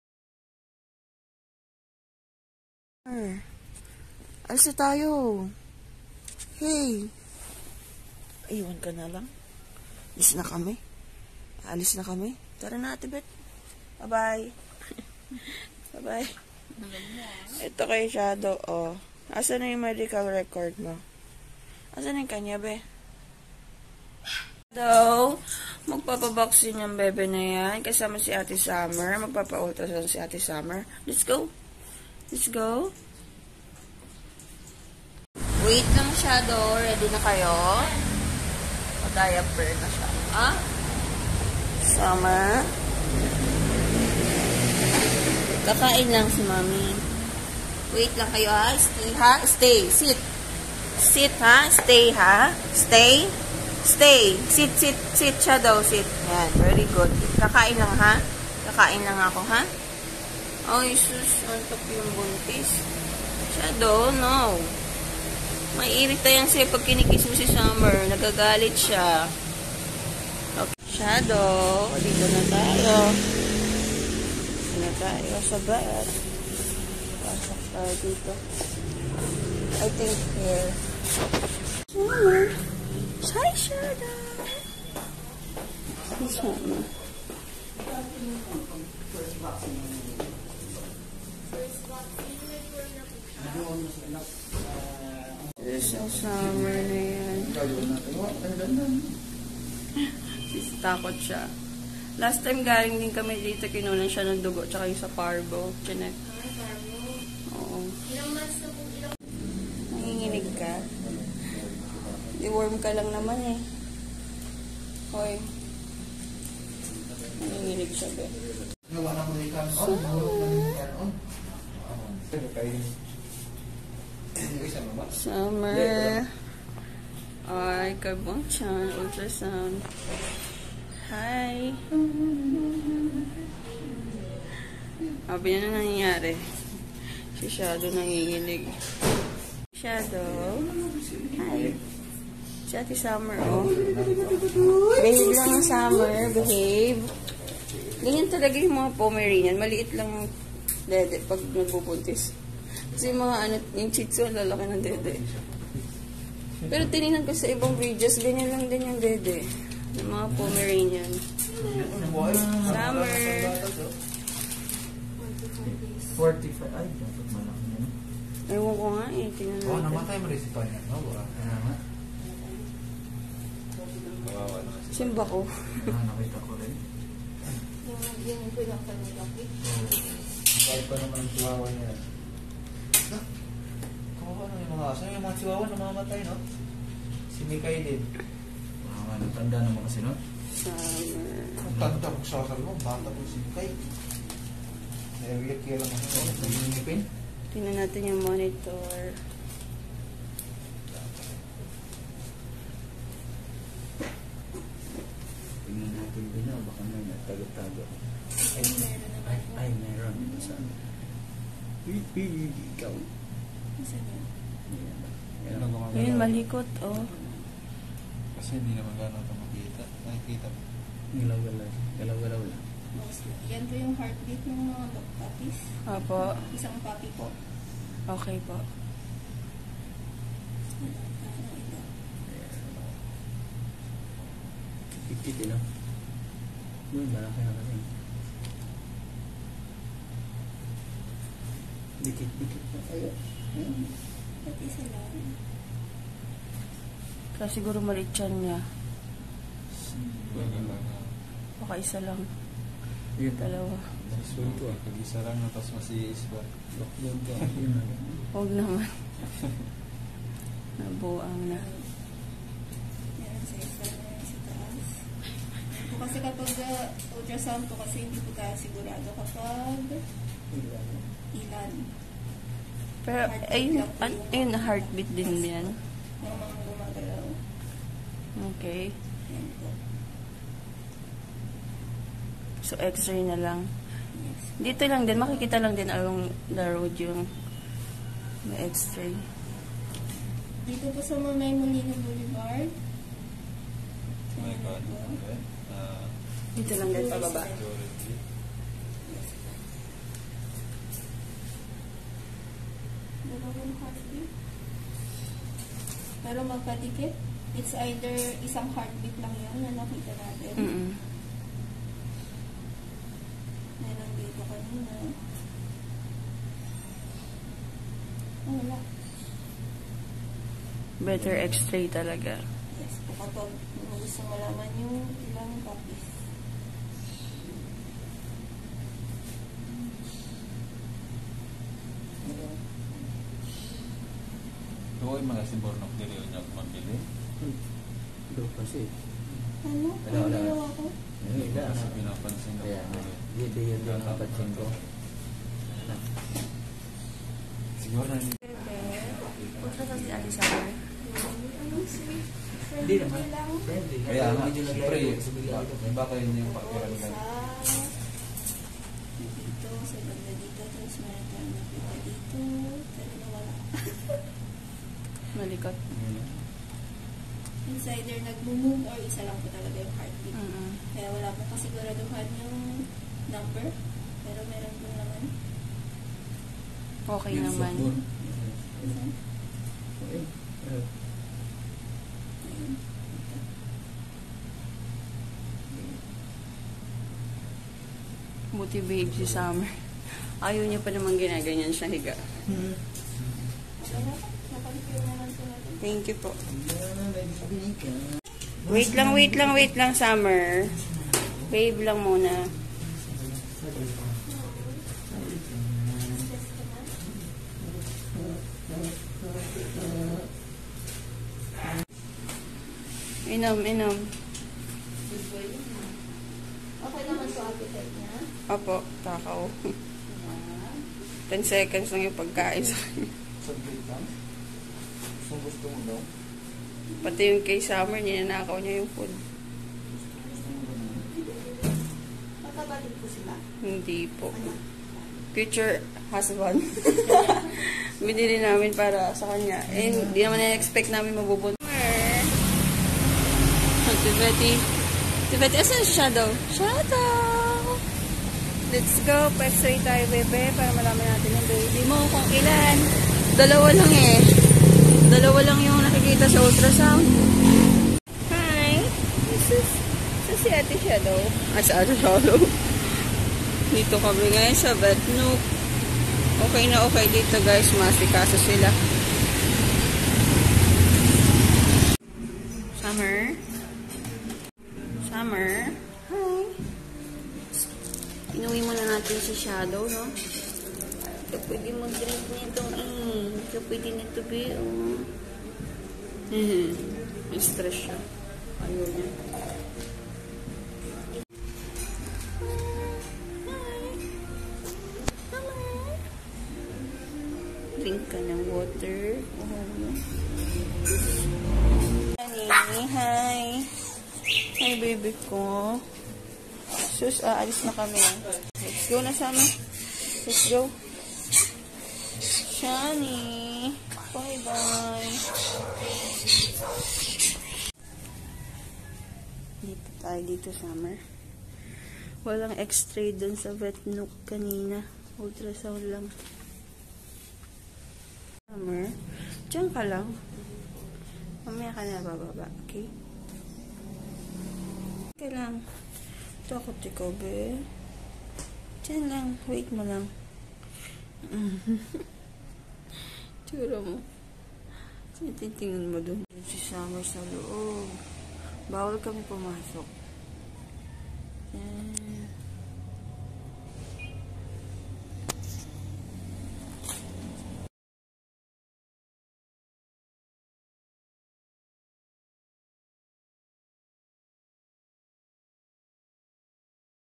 Summer's Ultrasound | Pomeranian | iT'S KITTY'S VLOG
1st Stud ni Summer False Pregnancy 🥺
2nd Period nya hoping na Meron ng Puppies 🙏🐾💕
Kaya sobrang excited akong ipa ultrasound sya at the same time kinakabahan 🥺